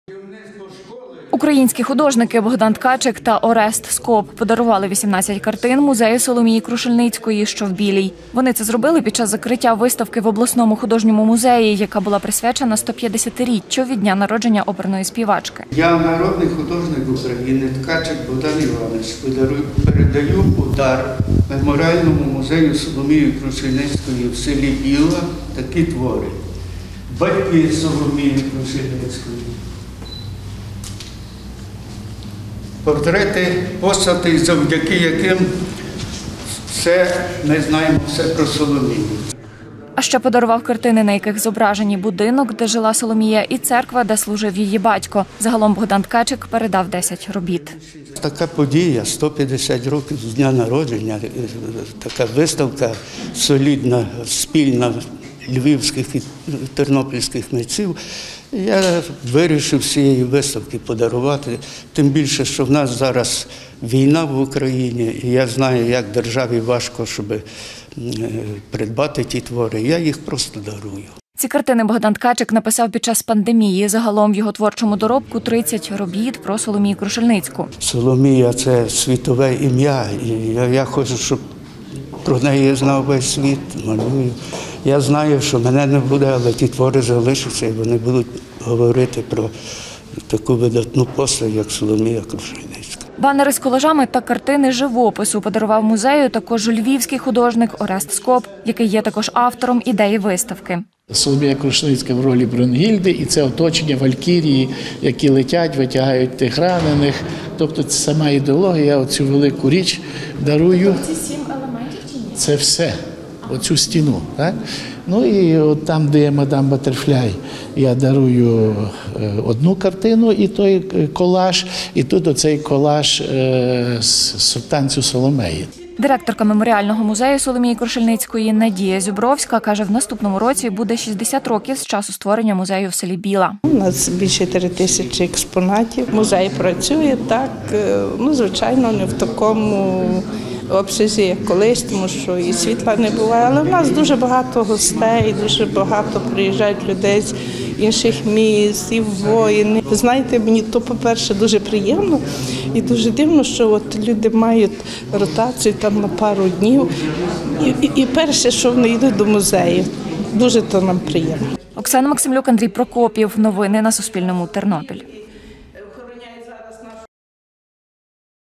Двоє художників подарували свої картини музею Соломії Крушельницької
Двоє українських художників – Богдан Ткачик та Орест Скоп подарували 18 картин музею Соломії Крушельницької, що в селі Біла Тернопільського району. Зробили це під час закриття виставки в обласному художньому музеї, яка була присвячена 150-річчю від дня народження оперної співачки.